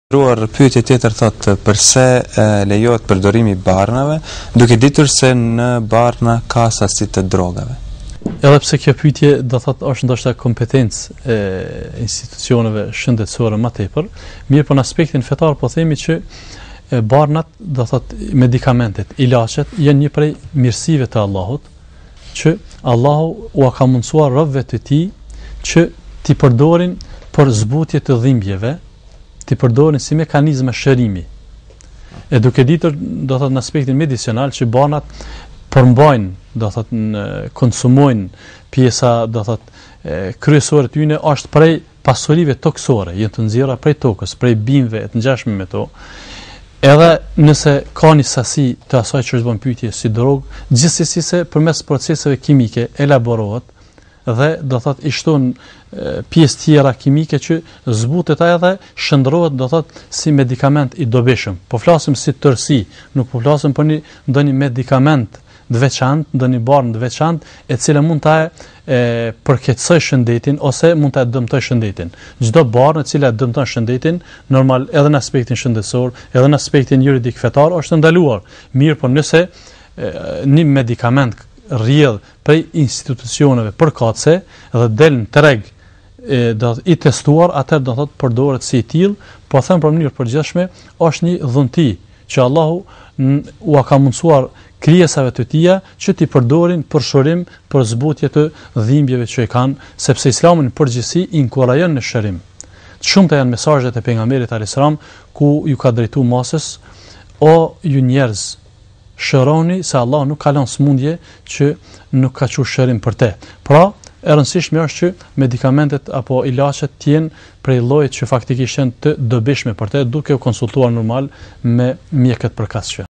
Përse lejohet përdorimi i barnave duke ditur se në barna ka sasi të drogës? - 115.7
Përse lejohet përdorimi i barnave duke ditur se në barna ka sasi të drogës?